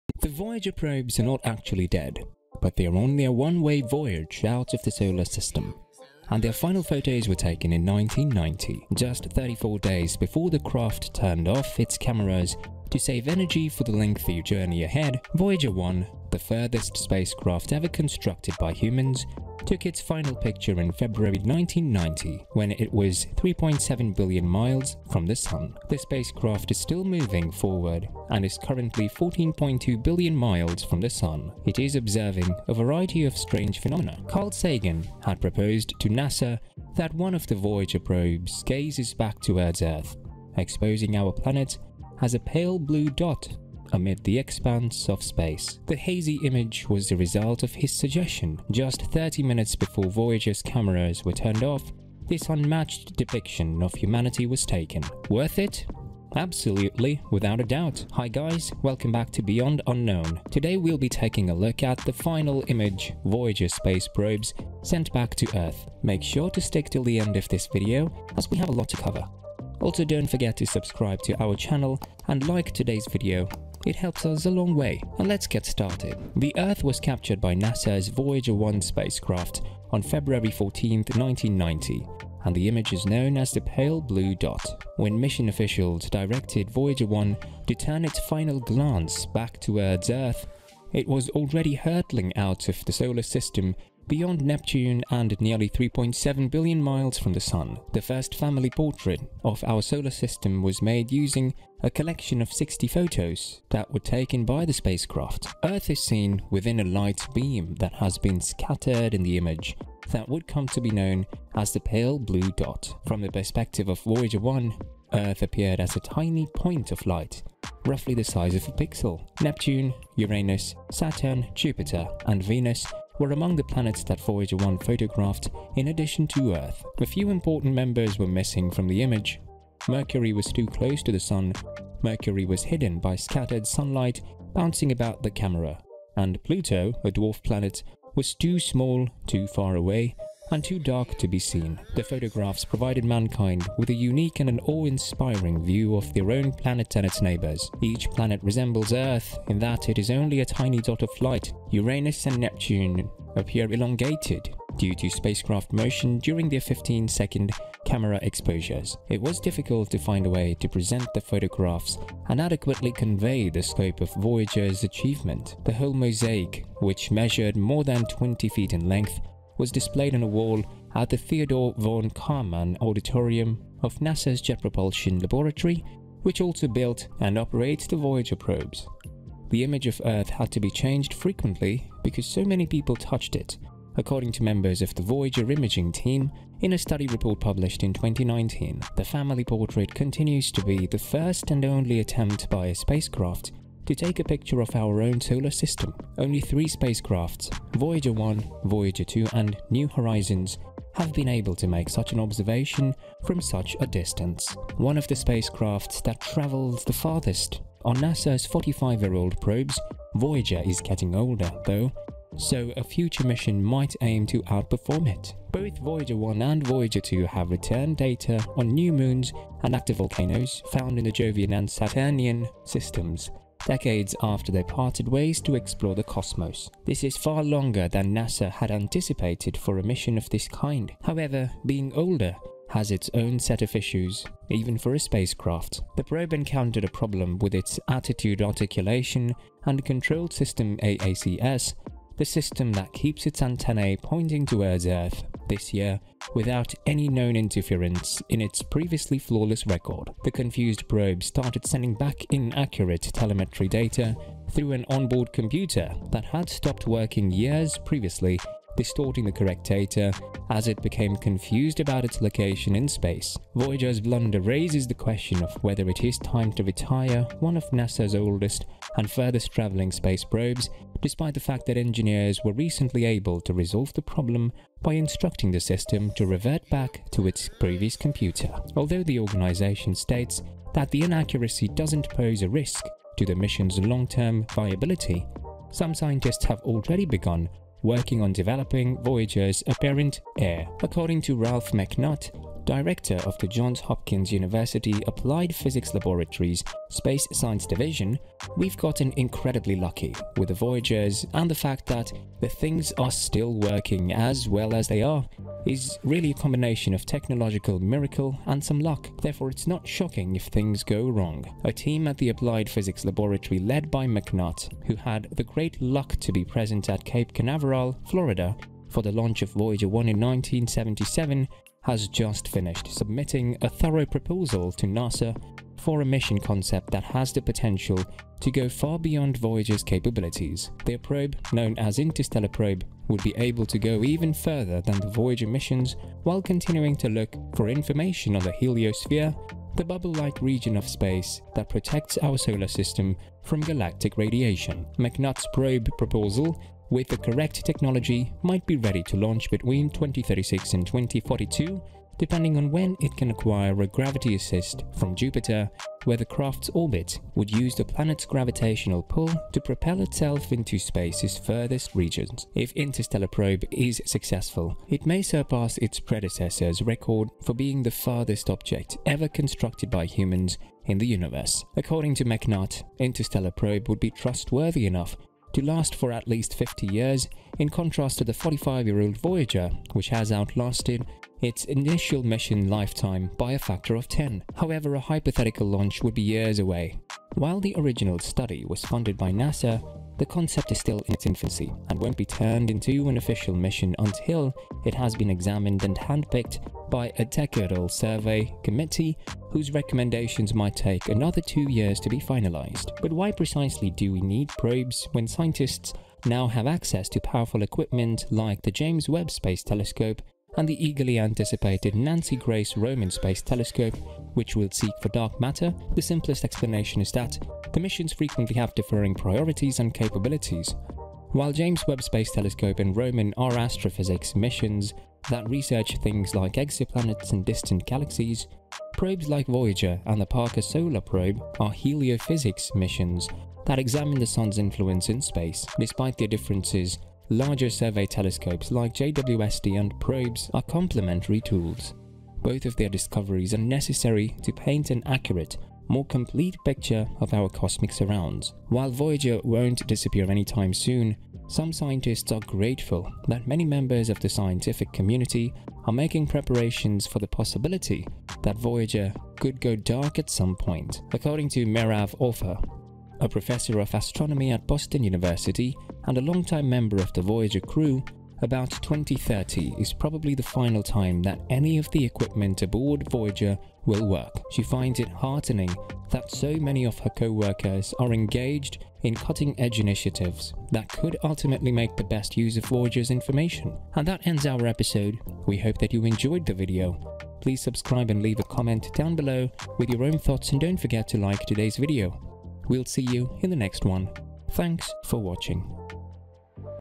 This NASA’s Space Probe SENT Its FINAL Image In Space And It Is Horrifying!
The Voyager probes are not actually dead, but they are on a one-way voyage out of the solar system, and their final photos were taken in 1990. Just 34 days before the craft turned off its cameras to save energy for the lengthy journey ahead, Voyager 1—the furthest spacecraft ever constructed by humans—took its final picture in February 1990, when it was 3.7 billion miles from the Sun. The spacecraft is still moving forward and is currently 14.2 billion miles from the Sun. It is observing a variety of strange phenomena. Carl Sagan had proposed to NASA that one of the Voyager probes gazes back towards Earth, exposing our planet as a "pale blue dot" amid the expanse of space. The hazy image was the result of his suggestion. Just 30 minutes before Voyager's cameras were turned off, this unmatched depiction of humanity was taken. Worth it? Absolutely without a doubt.

Hi guys! Welcome back to Beyond Unknown. Today we will be taking a look at the final image Voyager space probes sent back to Earth. Make sure to stick till the end of this video as we have a lot to cover.

The Earth was captured by NASA's Voyager 1 spacecraft on February 14, 1990, and the image is known as the Pale Blue Dot.

When mission officials directed Voyager 1 to turn its final glance back toward Earth, it was already hurtling out of the solar system, beyond Neptune and nearly 3.7 billion miles from the Sun. The first "family portrait" of our solar system was made using a collection of 60 photos that were taken by the spacecraft.

Earth is seen within a light beam that has been scattered in the image that would come to be known as the Pale Blue Dot. From the perspective of Voyager 1, Earth appeared as a tiny point of light, roughly the size of a pixel.

Neptune, Uranus, Saturn, Jupiter, and Venus were among the planets that Voyager 1 photographed in addition to Earth. A few important members were missing from the image: Mercury was too close to the Sun, Mercury was hidden by scattered sunlight bouncing about in the camera, and Pluto, a dwarf planet, was too small, too far away, and too dark to be seen.
The photographs provided mankind with a unique and awe-inspiring view of their own planet and its neighbors. Each planet resembles Earth in that it is only a tiny dot of light, Uranus and Neptune appear elongated due to spacecraft motion during their 15-second camera exposures.

It was difficult to find a way to present the photographs and adequately convey the scope of Voyager's achievement. The whole mosaic, which measured more than 20 feet in length, was displayed on a wall at the Theodore von Kármán Auditorium of NASA's Jet Propulsion Laboratory, which also built and operates the Voyager probes.

The image of Earth had to be changed frequently because so many people touched it, according to members of the Voyager imaging team in a study report published in 2019.

The family portrait continues to be the first and only attempt by a spacecraft to take a picture of our own solar system. Only three spacecraft, Voyager 1, Voyager 2, and New Horizons, have been able to make such an observation from such a distance.

One of the spacecrafts that travels the farthest are NASA's 45-year-old probes. Voyager is getting older, though, so a future mission might aim to outperform it.

Both Voyager 1 and Voyager 2 have returned data on new moons and active volcanoes found in the Jovian and Saturnian systems, decades after they parted ways to explore the cosmos. This is far longer than NASA had anticipated for a mission of this kind. However, being older has its own set of issues, even for a spacecraft.